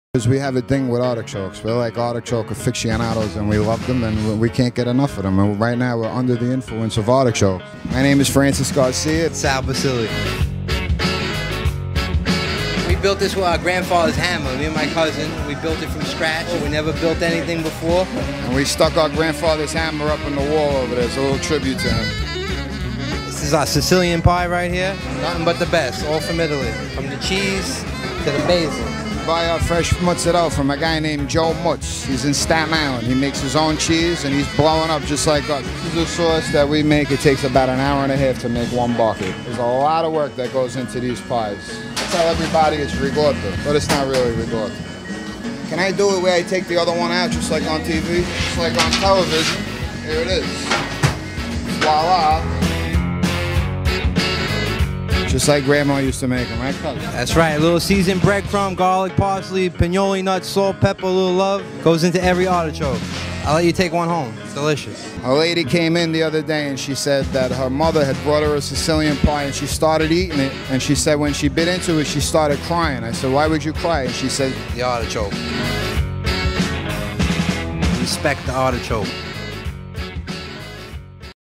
See what goes into an Artichoke Basille pie
When cousins Sal Basille and Francis Garcia were kids, they'd drop their backpacks in the back room of their family's restaurant, Basille's on Forest Avenue in West Brighton, throw on an apron, and "get to work."
Since they grew up making pizza, it's no surprise they now own 12 restaurants  across New York City, California and Florida.

And now, work on their 13th restaurant -- to be located in Empire Outlets, New York City's first outlet mall being build on the St. George waterfront -- is about to commence.

And they plan to bring back many of the favorite menu items their loyal patrons remember from Basille's. (Courtesy of Artichoke Basille’s Pizza)